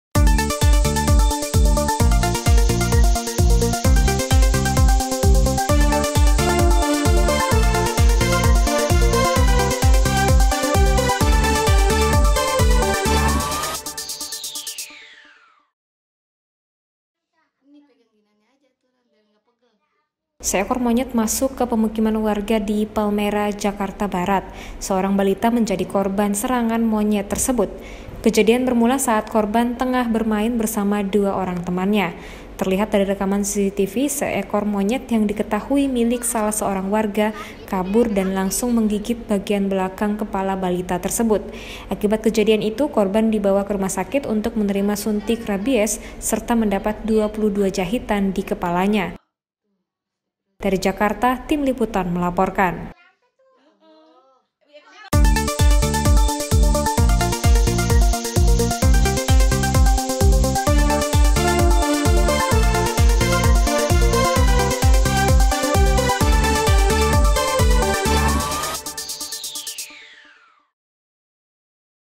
SEORANG BALITA DI PALMERAH JADI KORBAN SERANGAN MONYET MENDAPAT 22 JAHITAN
Seekor monyet masuk ke pemukiman warga di Palmerah, Jakarta Barat. Seorang balita menjadi korban serangan monyet tersebut.

Kejadian bermula, saat korban yang tengah bermain bersama dua orang temannya. Terlihat dari rekaman cctv, seekor monyet yang diketahui milik salah seorang warga, kabur dan langsung menggigit bagian belakang kepala balita tersebut.

Akibat kejadian itu, korban dibawa kerumah sakit untuk menerima suntik rabies serta mendapat 22 jahitan di kepalanya.

Melaporkan.